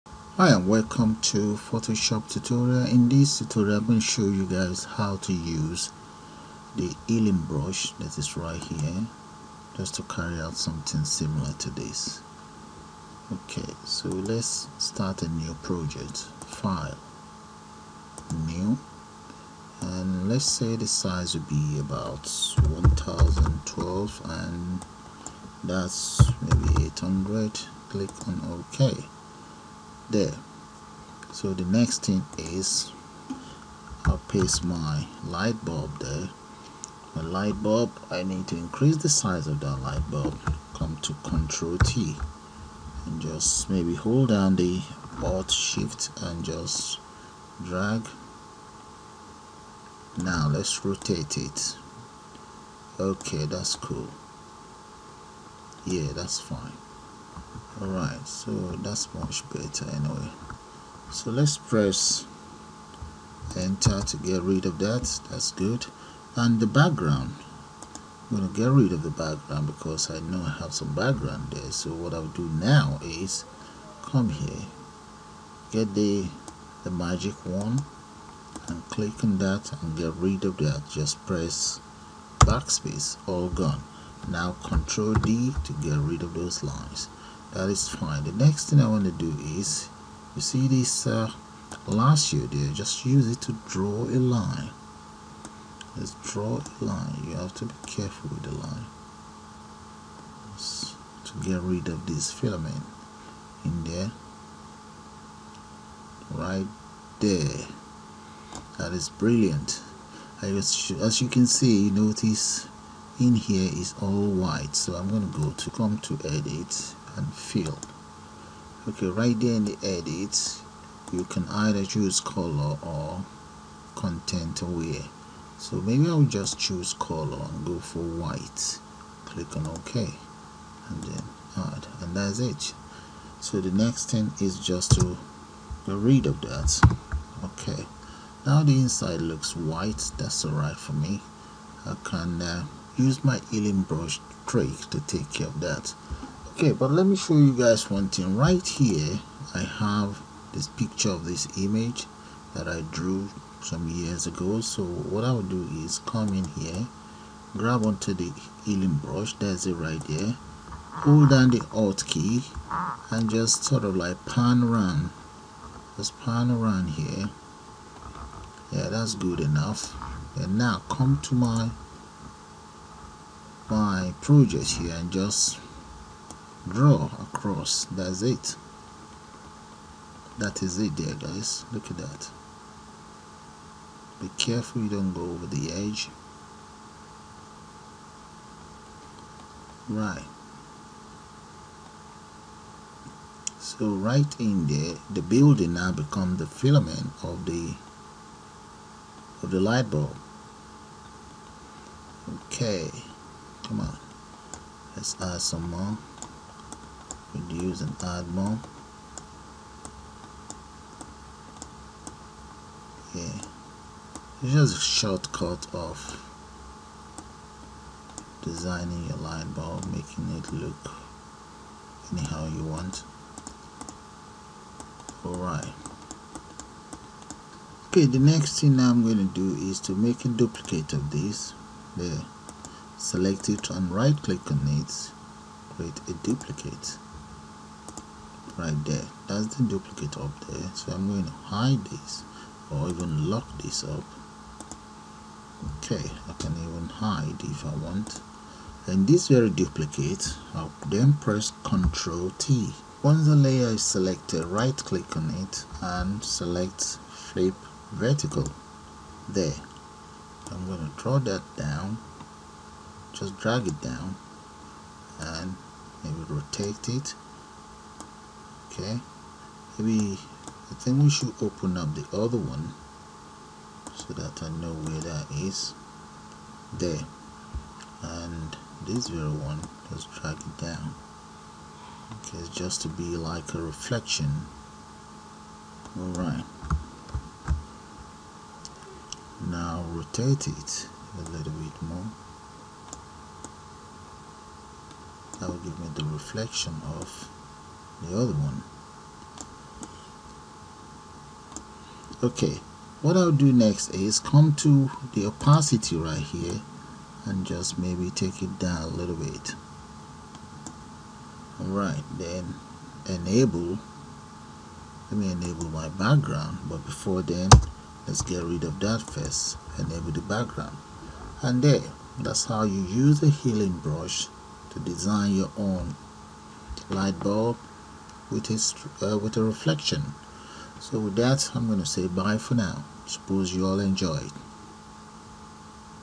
How to use the Healing Brush Tool in Photoshop CC
Using Healing Brush Tools in Photoshop CC 2015.5